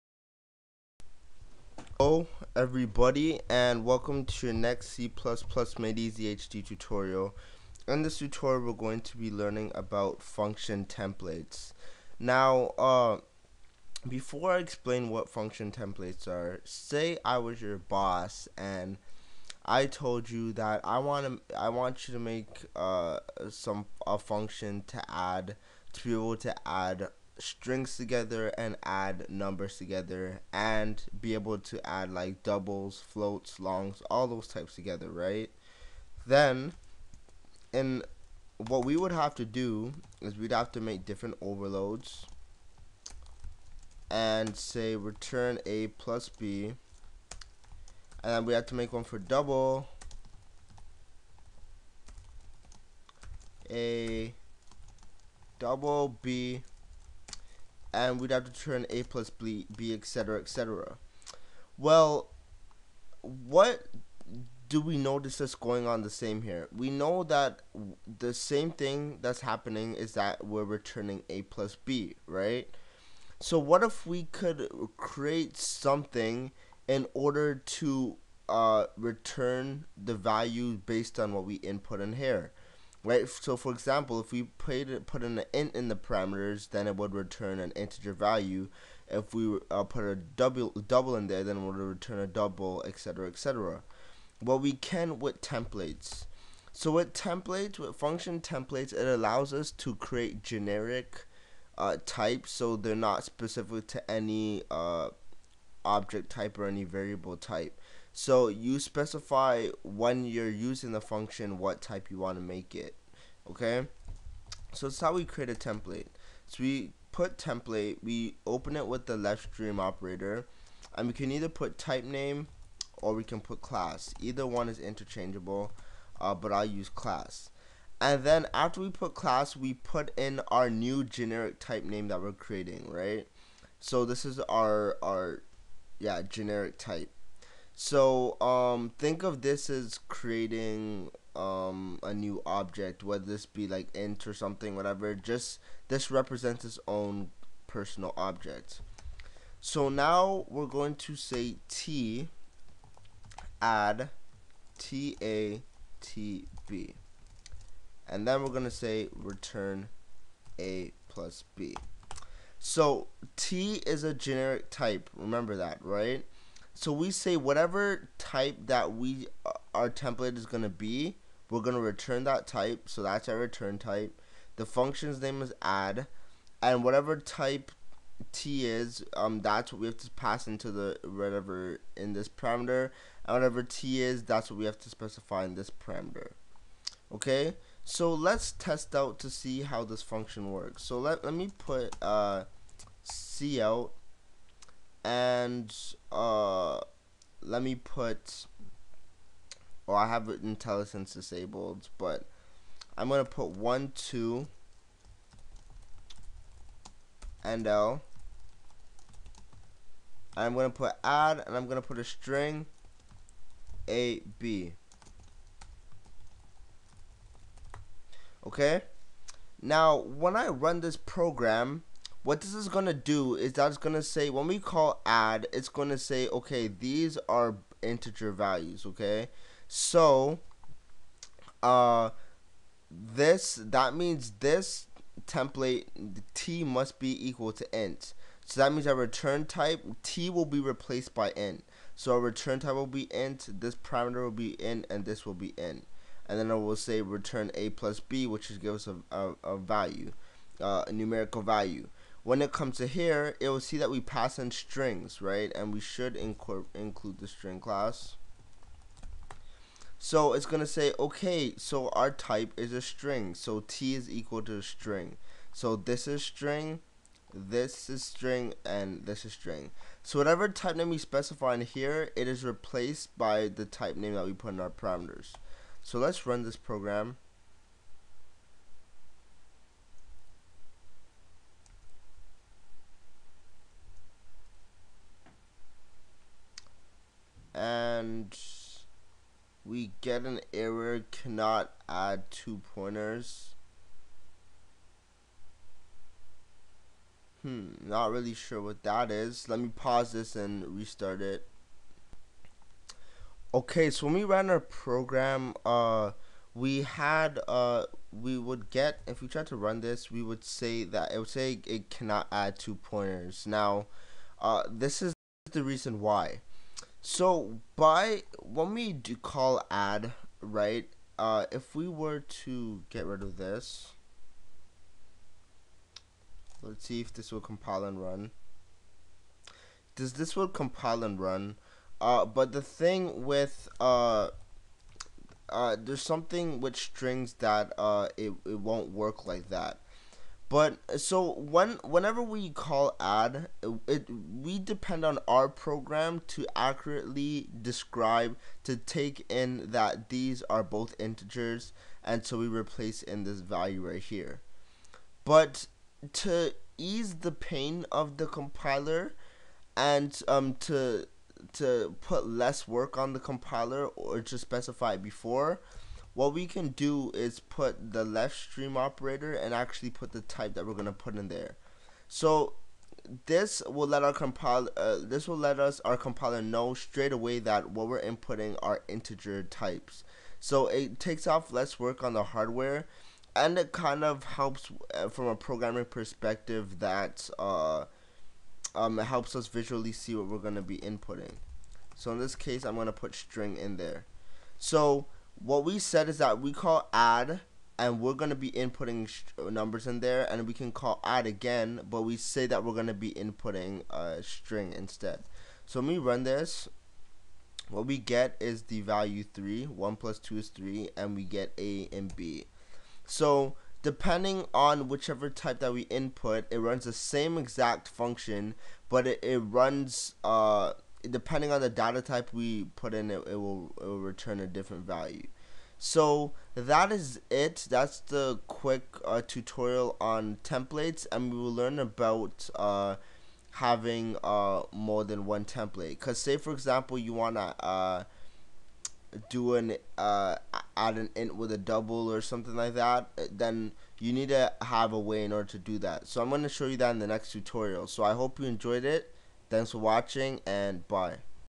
C++ Made Easy HD Tutorial 40 - Function Templates
In this tutorial we will be learning on how to create generic types in c++ using templates. Templates are often seen as complicated and hard but over the span of the next few tutorials you will learn how to use templates in and out in an easy manner. Hope you enjoy :).